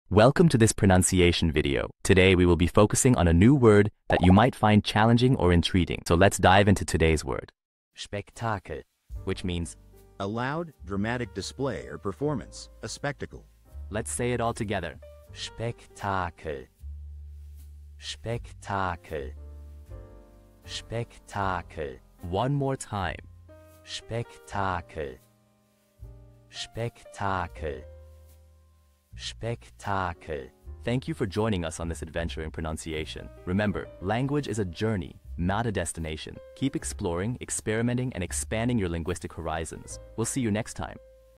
How to pronounce Spektakel
Master the Pronunciation of 'Spektakel which Means spectacle' - which means : A loud, dramatic display or performance;a Spectacle. 📝 with @PronunciationGerman 🎓🗣️ - [Your Guide to German]

Learn to pronounce 'Spektakel' perfectly in German with our easy and engaging tutorial by @PronunciationGerman! 📖📽️ In this comprehensive video, we provide you with native speaker examples 🗣️👂 and phonetic breakdowns 🎵 to ensure you get the pronunciation just right.

How to say spectacle in German